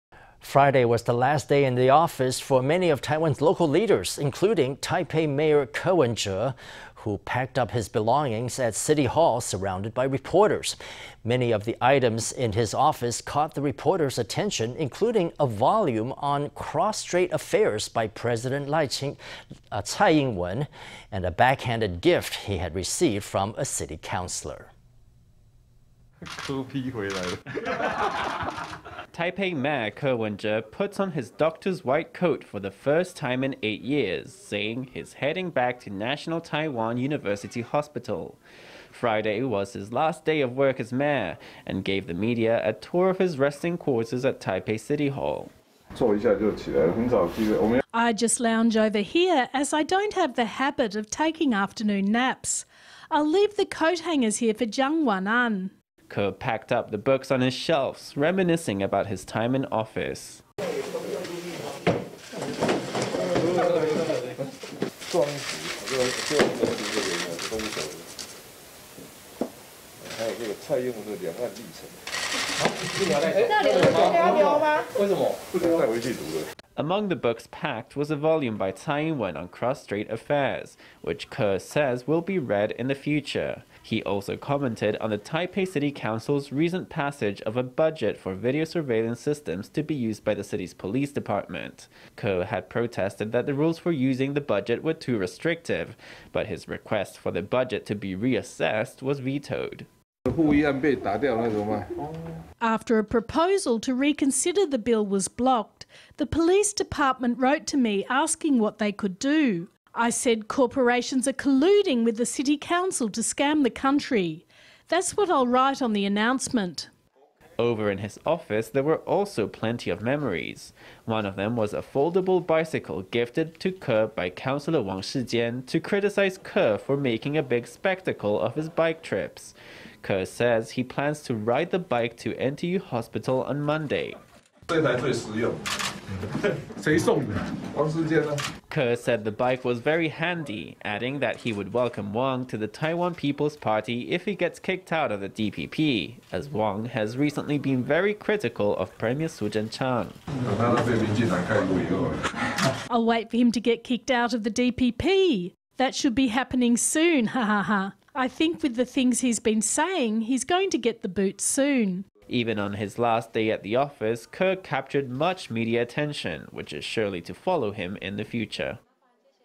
Taipei Mayor Ko packs up belongings on his last day of work at city hall
Friday was the last day in the office for many of Taiwan’s local leaders, including Taipei Mayor Ko Wen-je, who packed up his belongings at city hall surrounded by reporters. Many of the items in his office caught the reporters’ attention, including a volume on cross-strait affairs by President Tsai Ing-wen and a backhanded gift he had received from a city councilor.

Taipei Mayor Ko Wen-je puts on his doctor’s white coat for the first time in eight years, saying he’s heading back to National Taiwan University Hospital. Friday was his last day of work as mayor, and gave the media a tour of his resting quarters at Taipei City Hall.

Ko Wen-je
Taipei mayor
I just lounge over here, as I don’t have the habit of taking afternoon naps. I’ll leave the coathangers here for Chiang Wan-an.

Ko packed up the books on his shelves, reminiscing about his time in office.

Among the books packed was a volume by Tsai Ing-wen on cross-strait affairs, which Ko says will be read in the future. He also commented on the Taipei City Council’s recent passage of a budget for video surveillance systems to be used by the city’s police department. Ko had protested that the rules for using the budget were too restrictive, but his request for the budget to be reassessed was vetoed.

Ko Wen-je
Taipei mayor
After a proposal to reconsider the bill was blocked, the police department wrote to me asking what they could do. I said, “Corporations are colluding with the city council, to scam the country.” That’s what I’ll write on the announcement.

Over in his office, there were also plenty of memories. One of them was a foldable bicycle gifted to Ko by Councilor Wang Shih-chien to criticize Ko for making a big spectacle of his bike trips. Ko says he plans to ride the bike to NTU Hospital on Monday.

Ko said the bike was very handy, adding that he would welcome Wang to the Taiwan People’s Party if he gets kicked out of the DPP, as Wang has recently been very critical of Premier Su Tseng-chang.

Ko Wen-je
Taipei mayor
I’ll wait for him to get kicked out of the DPP. That should be happening soon, hehehe. I think with the things he’s been saying, he’s going to get the boot soon.

Even on his last day at the office, Ko captured much media attention, which is surely to follow him in the future.